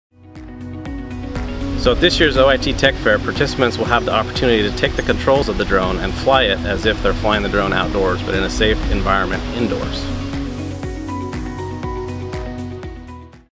OIT Tech Fair Drone Simulation
Corey Cardoza tells students, faculty and staff what to look forward to at the 15th Annual OIT Tech Fair.